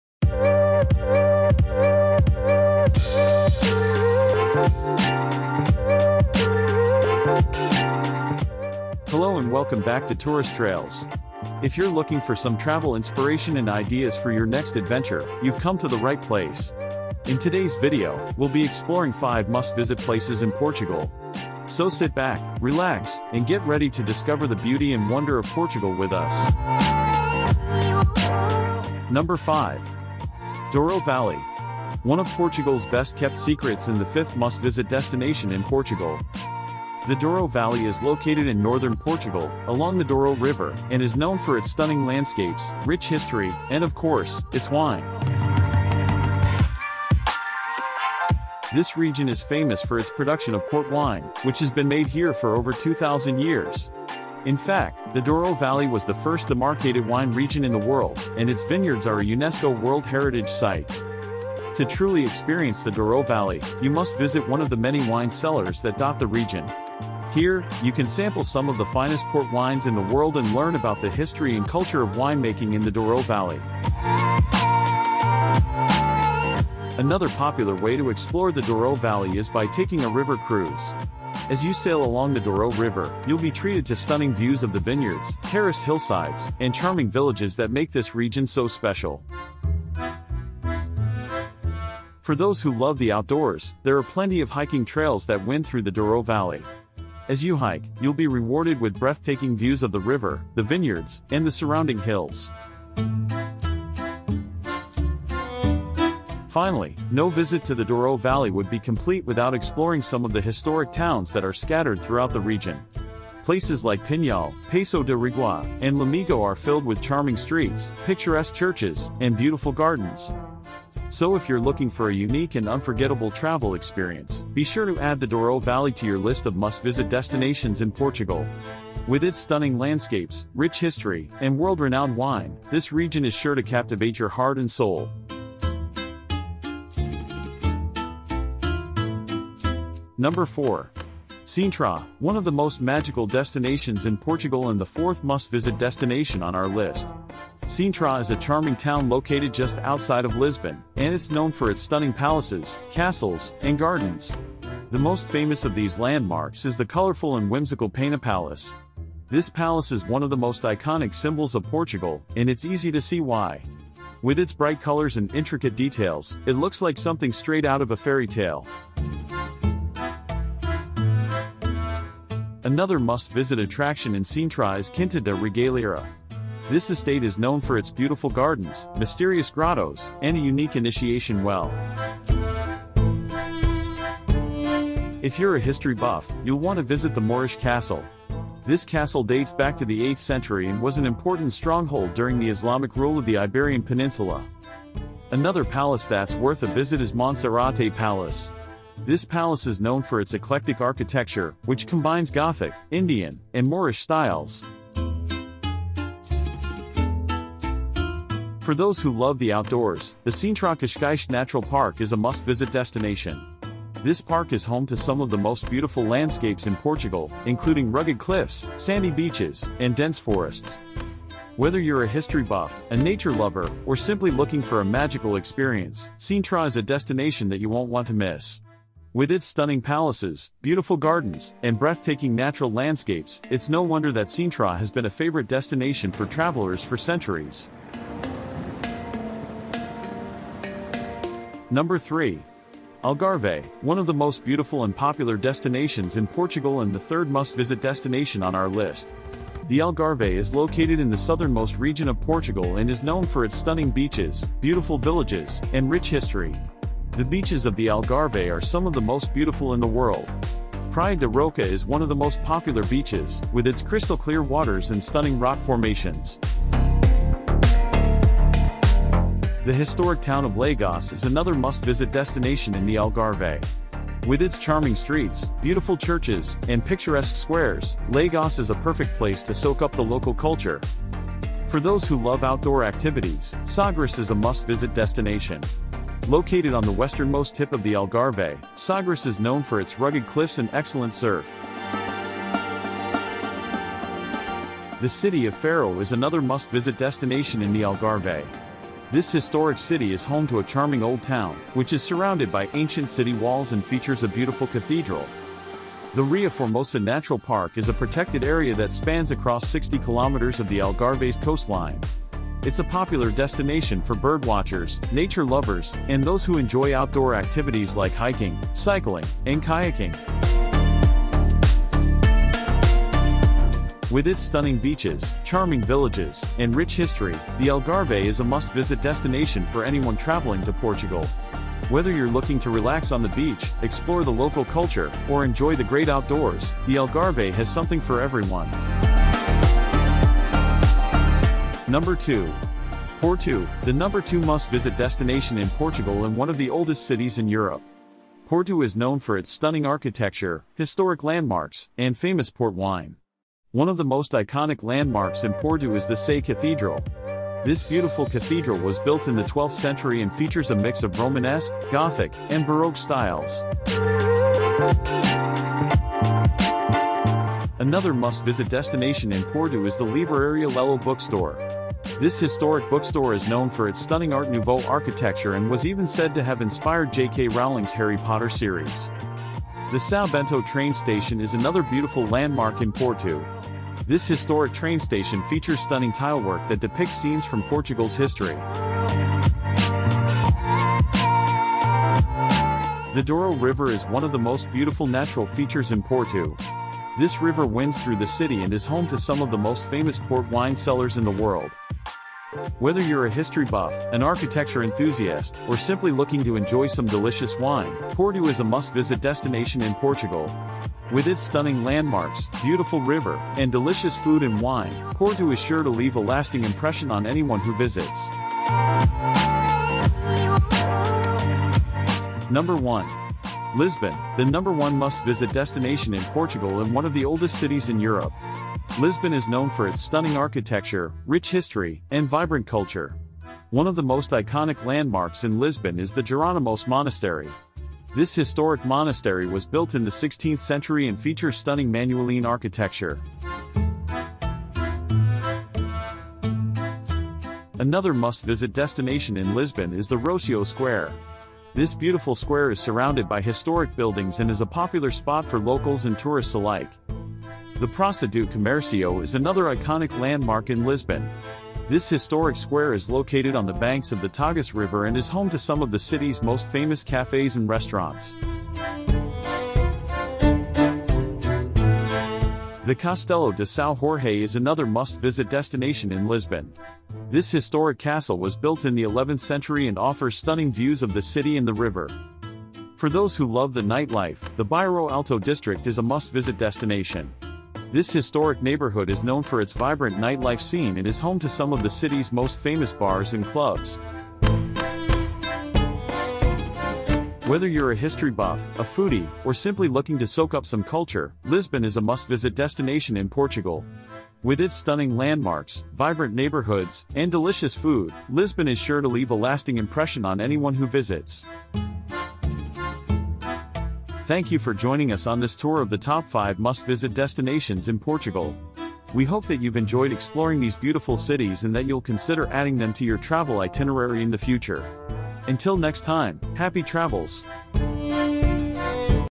5 Must Visit Places In Portugal | Portugal Travel Guide
5 Must Visit Places In Portugal | Portugal Travel Guide

In this video, we'll take you on a journey through the top five must-visit destinations in Portugal. From the historic city of Porto and the enchanting town of Sintra to the stunning beaches of the Algarve and the vibrant capital of Lisbon, we'll show you the best of what Portugal has to offer. Join us as we uncover hidden gems, sample delicious local cuisine, and soak up the rich culture and history of this beautiful country. Whether you're planning your next vacation or simply looking for some travel inspiration, this video is sure to leave you dreaming of the sunny shores and picturesque landscapes of Portugal.